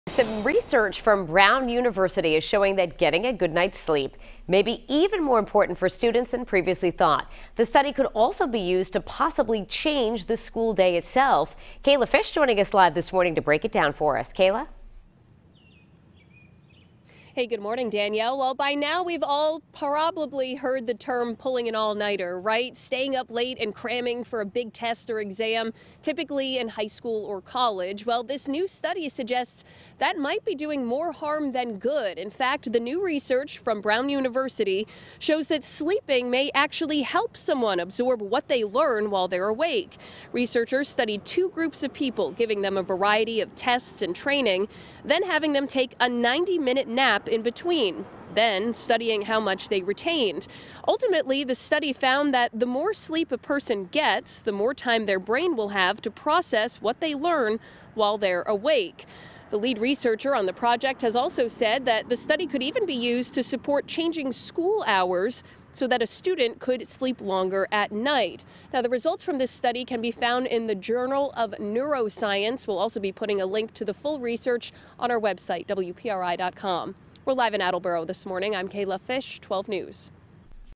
Study: Sleep may be key to better memory
Research from Brown University shows that getting a good night's sleep may be even more important for students than previously thought.